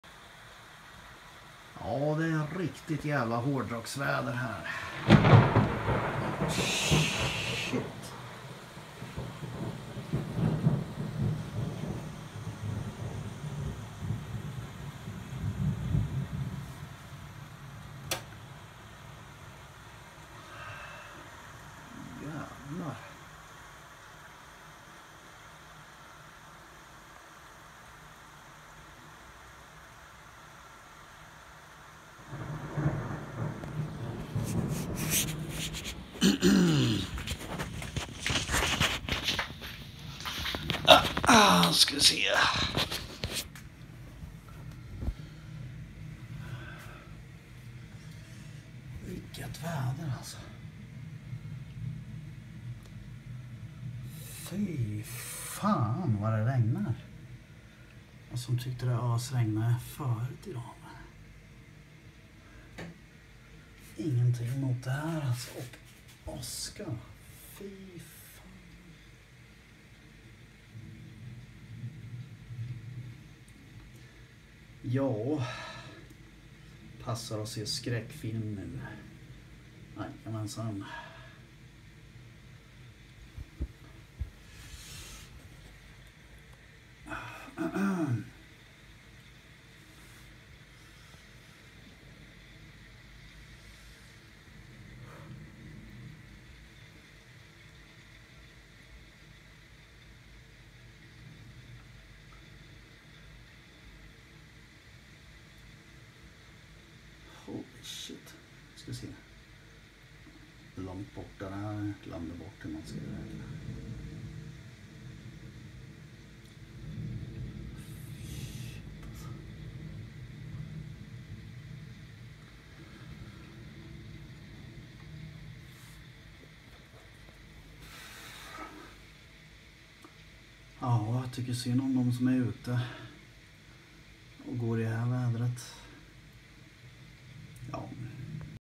!!!HÅRDROCKSVÄDER!!!
Regn och åskovädret söndagen den 6 Augusti 2023